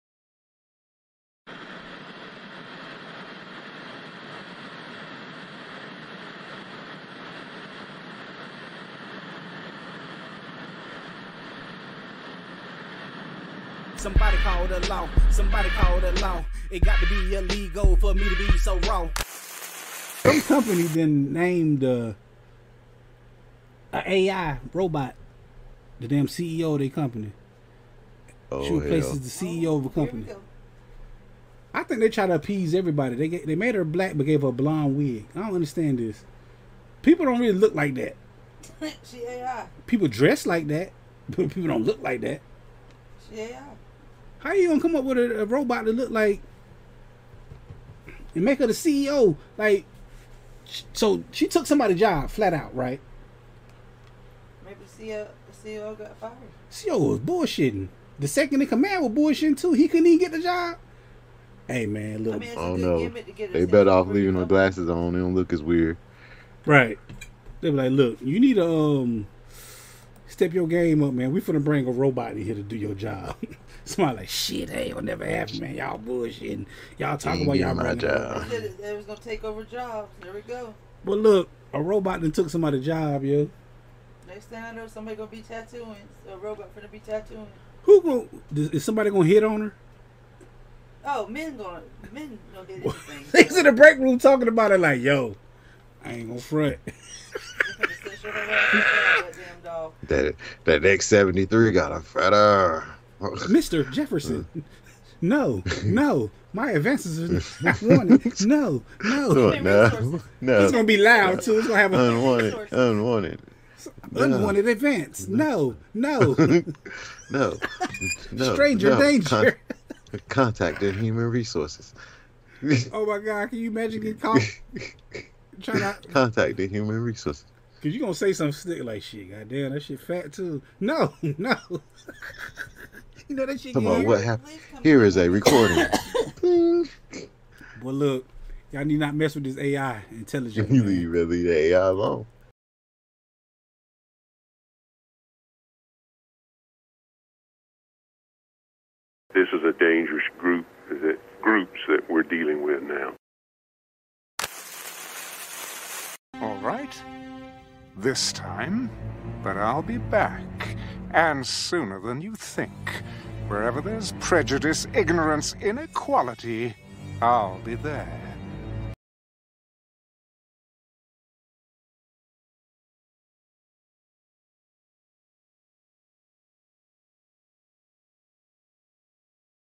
MIKA World's First AI CEO? Are Robots Stealing Jobs?
Drink company appoints the worlds first AI CEO...
Maaaaaaan!!!